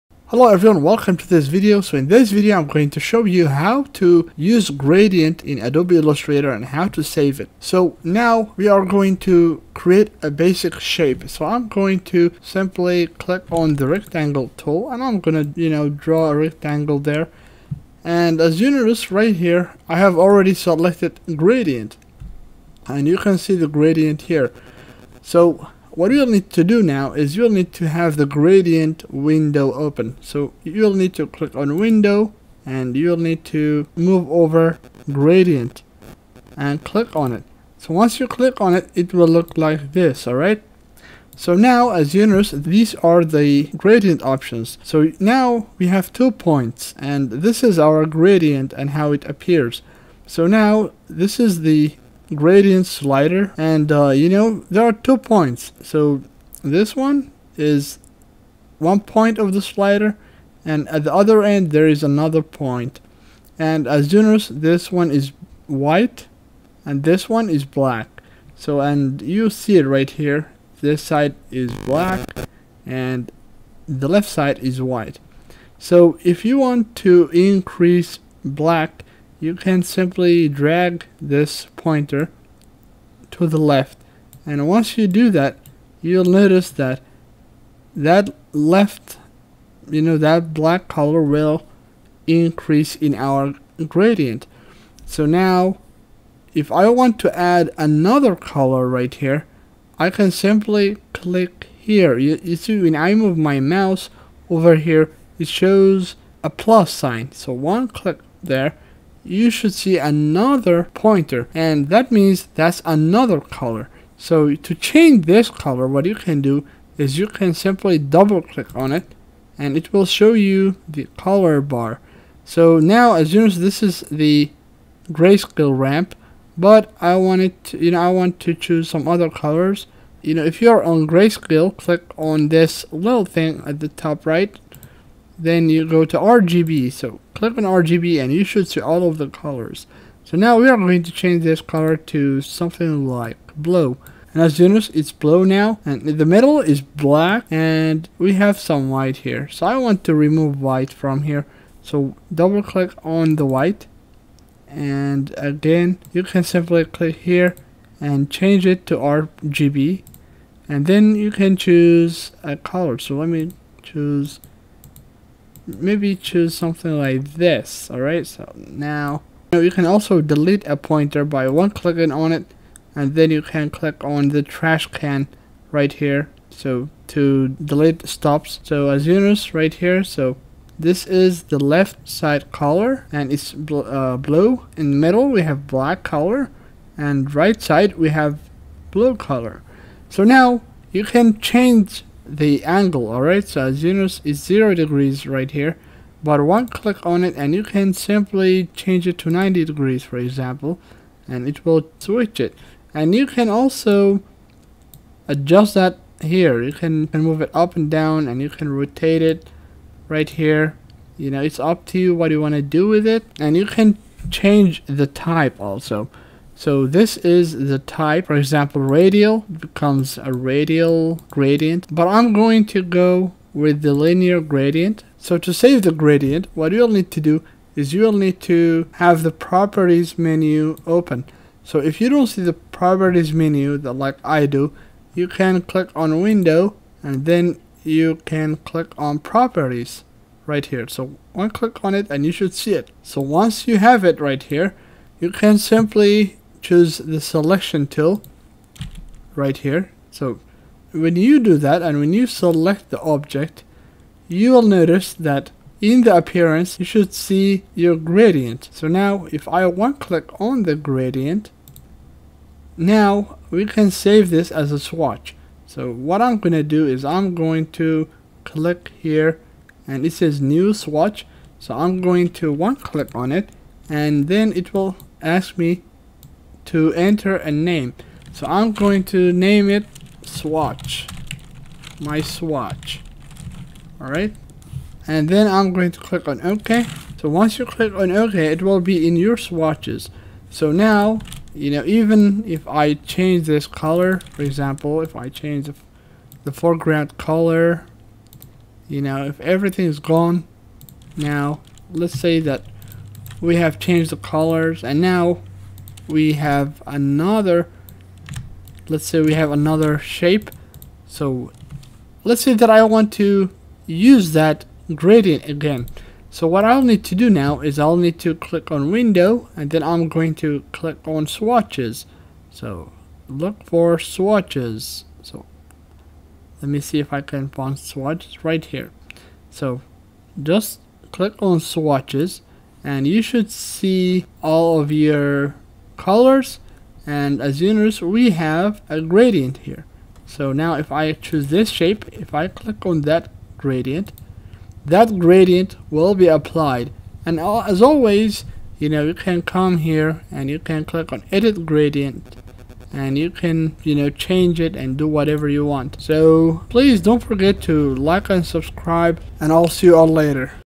How to create and save gradients in Adobe illustrator
in this video I will show you how to create gradients and how to save your gradient in adobe illustrator 2020